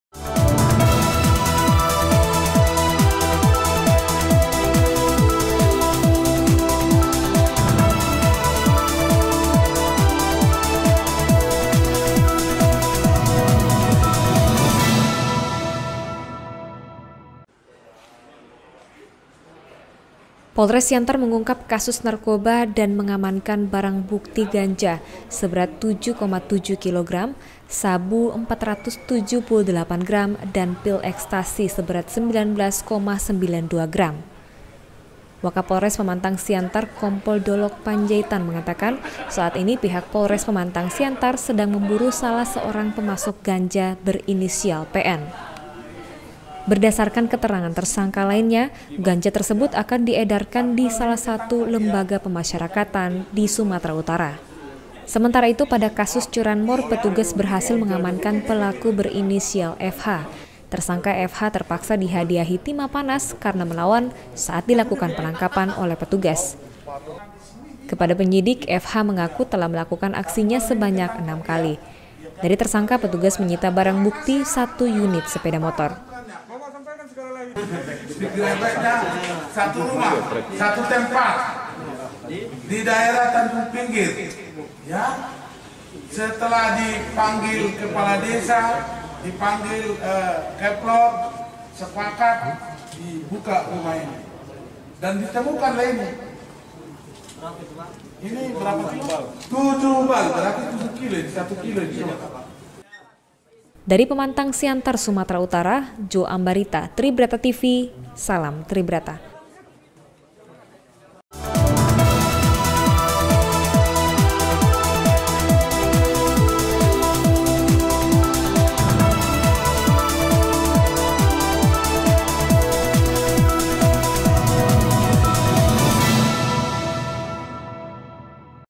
POLRES SIANTAR TANGKAP PELAKU NARKOBA DAN CURANMOR
Polres Siantar mengungkap kasus narkoba dan  mengamankan barang bukti ganja seberat 7,7 kilogram, sabu 478 gram/dan pil ekstasi seberat 19,92 gram.

Wakapolres Pematangsiantar Kompol Dolok Panjaitan mengatakan saat ini pihak polres Pematangsiantar sedang memburu salah seorang pemasok ganja berinisial PN. Dan berdasarkan keterangan tersangka lainnya ganja tersebut akan diedarkan ke salah satu lembaga pemasyarakatan di Sumatera Utara.

Sementara itu pada kasus curanmor petugas berhasil mengamankan pelaku berinisial FH. Tersangka FH terpaksa dihadiahi timah panas/karena melawan saat dilakukan penangkapan oleh petugas. Kepada penyidik FH mengaku telah melakukan aksinya sebanyak enam kali.
Dari tersangka petugas menyita barang bukti  satu unit sepeda motor.

Dari Pematangsiantar Sumatera Utara Joe Ambarita tribrata tv salam tribrata.